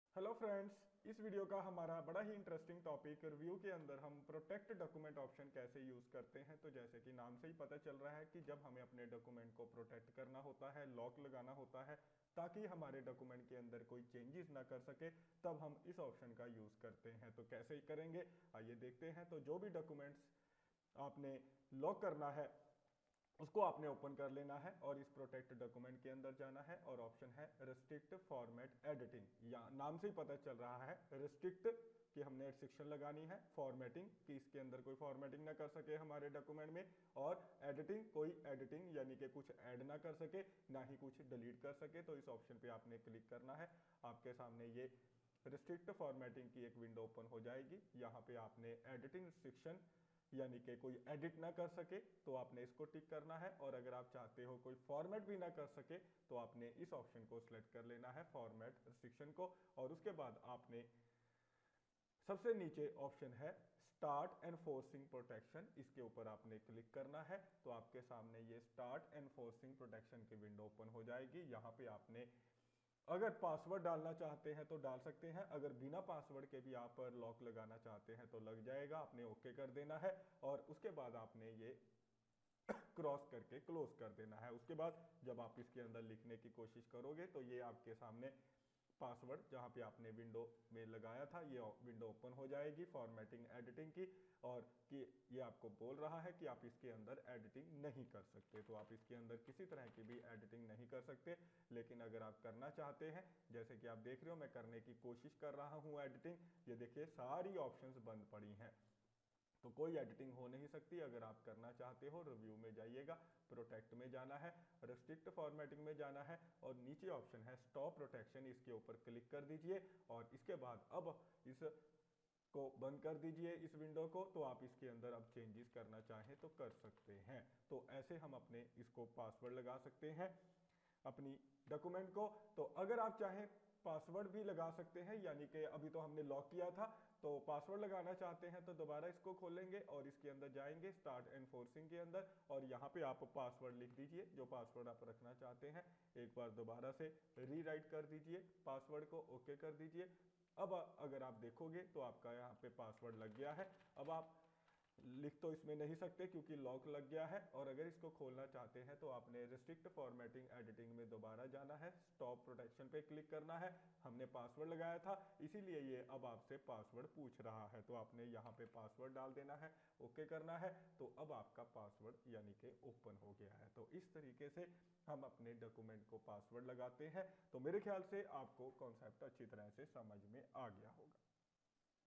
Protect Document Option - How to use MS Word
MS Word Options - How to use in MS Word 2017, 2016, 2013, 2010, 2007

In this video, we've covered "MS Word Options “Protect Document Option”
Which are used as follow?
This Option is used to protect the document to make changes in it by using this command. You can enter the password or without password Lock the document.